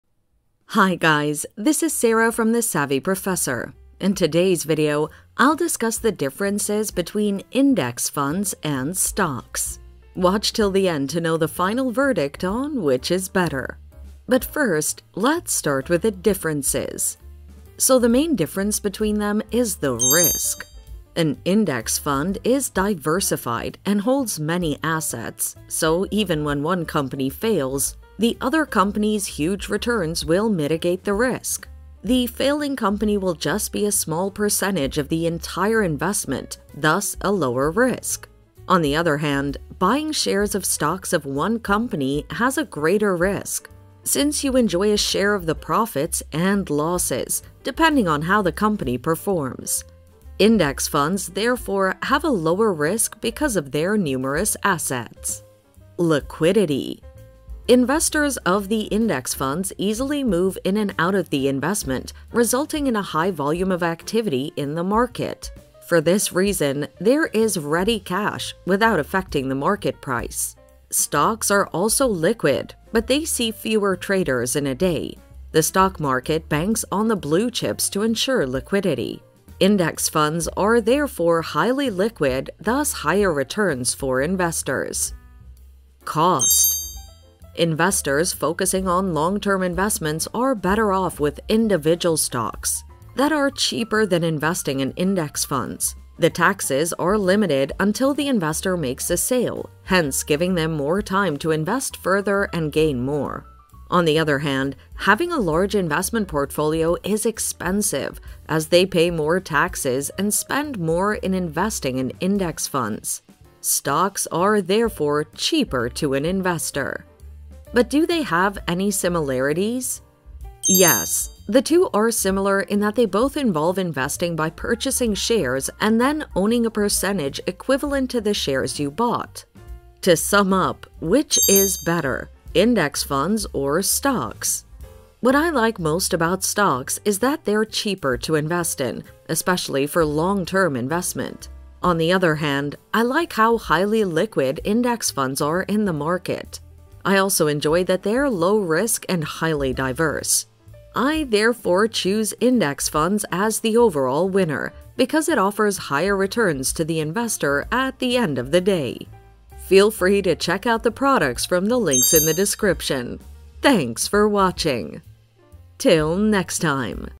Index Funds vs Stocks- What's the Difference? (Which is Better?)
Index Funds vs Stocks- What's the Difference? (Which is Better?). In this video, I will discuss the differences between Index Funds and Stocks.

The main difference between them is the risk
An index fund is diversified and holds many assets, so even when one company fails, the other companies’ huge returns will mitigate the risk. The failing company will just be a small percentage of the entire investment thus a lower risk. On the other hand, buying shares of stocks of one company has a greater risk since you enjoy a share of the profits and losses depending on how the company performs.